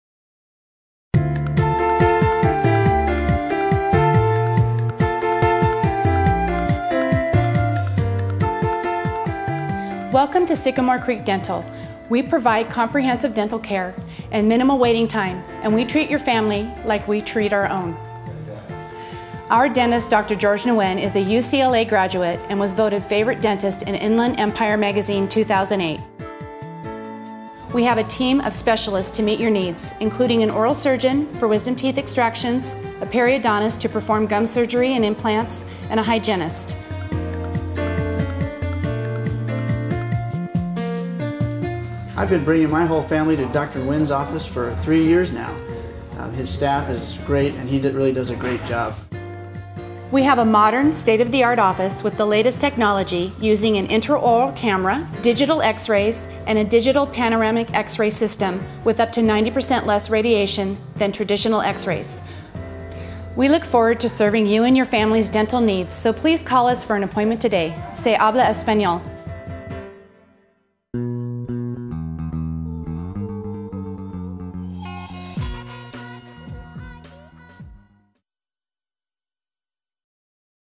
Sycamore Creek Dental Corona CA Oral Surgeons
Whiter teeth? Wisdom teeth extraction? Root canal? Sycamore Creek Dental in Corona, California, is your one-stop dental shop. Dr. George Nguyen and his team of specialists, working with state-of-the-art equipment, offer dental implants, veneers, oral surgery, gum treatments, fillings, and smile makeovers for everyone from grade school students to grandparents.